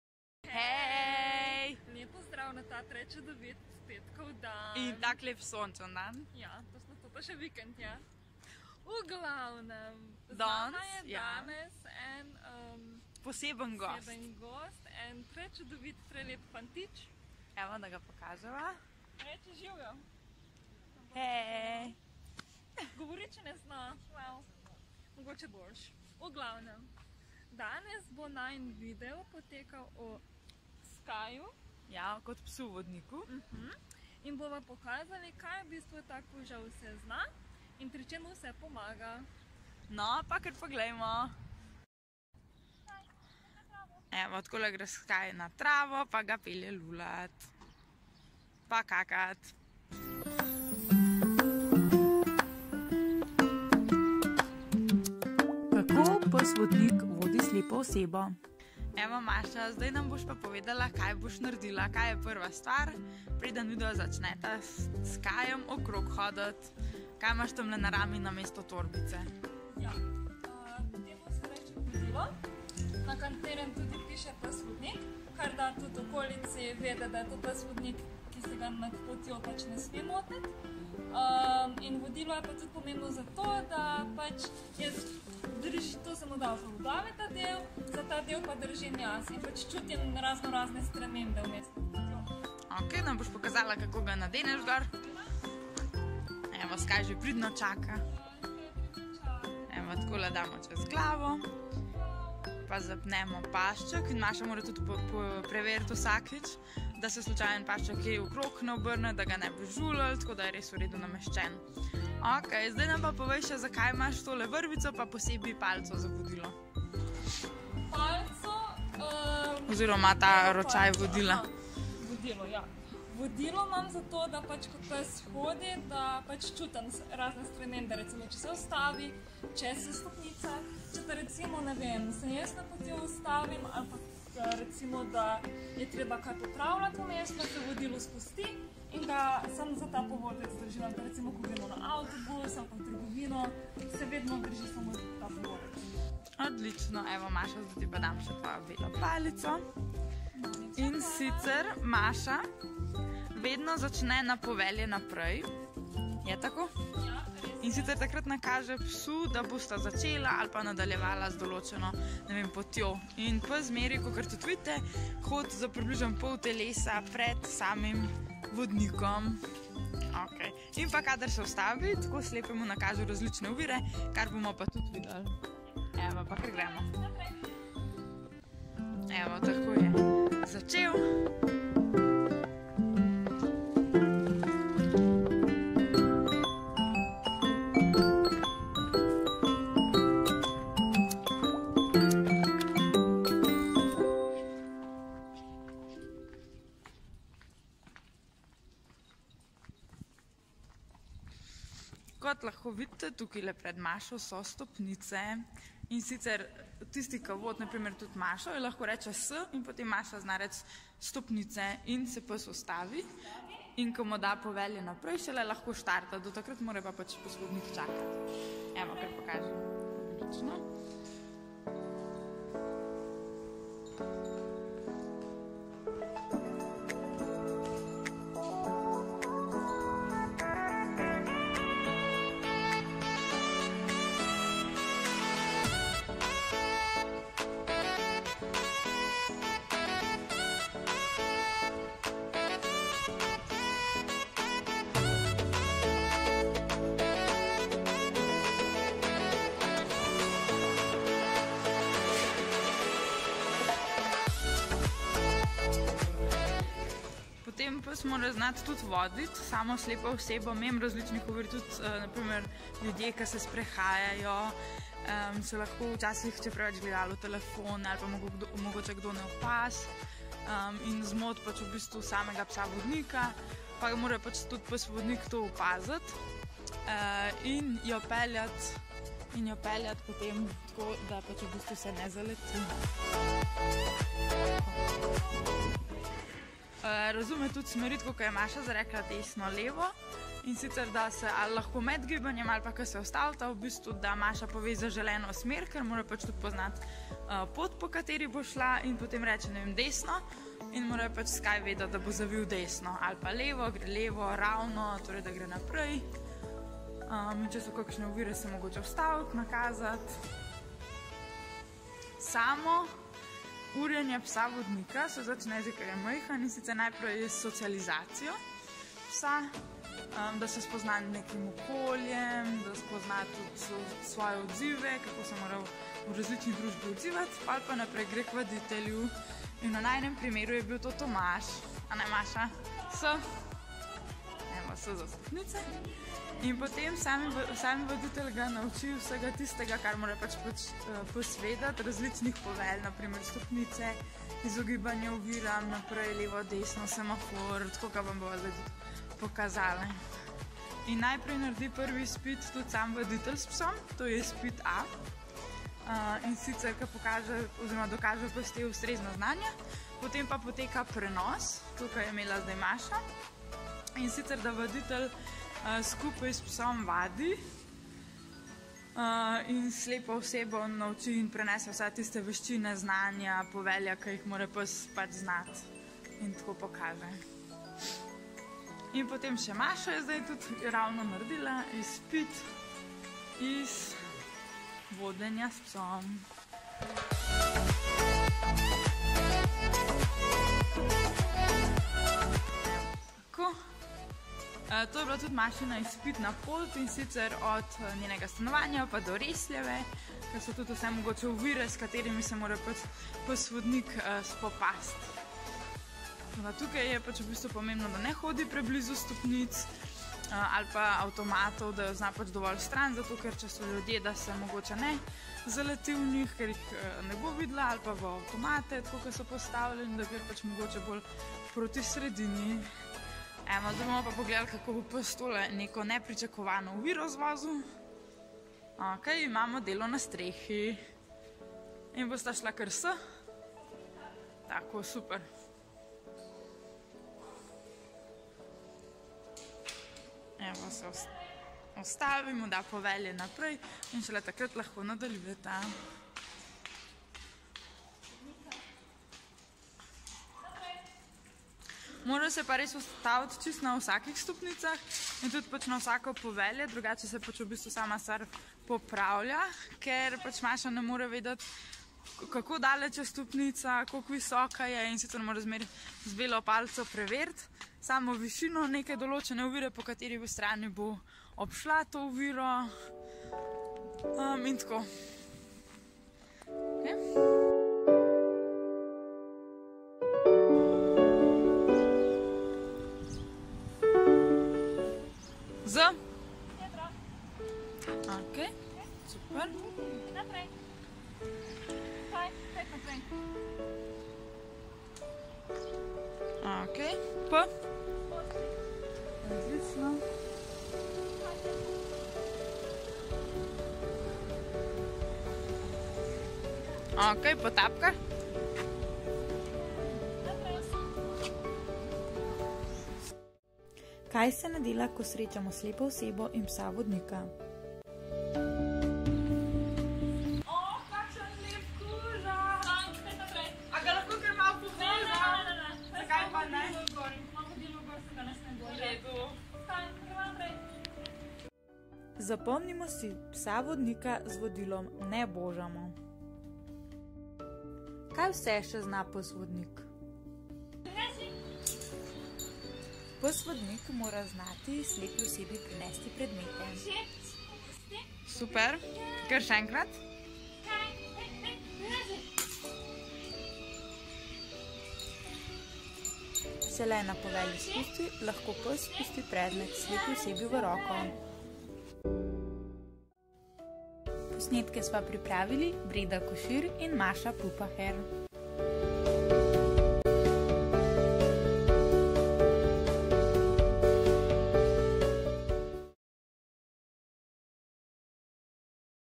VLOG 3: Vodnik slepih na štirih tačkah (Breda in Maša)
Ali ste se že srečali osebo, ki ni peljala psa na sprehod ampak on njo? Če vas zanima kako se to obnese v praksi, si oglejte video. V njem izveste več o pomoči štirinožnih prijateljev osebi s slepoto. :)

Avtor videoposnetka: Breda Košir
Oglejte si še ostale najine vloge: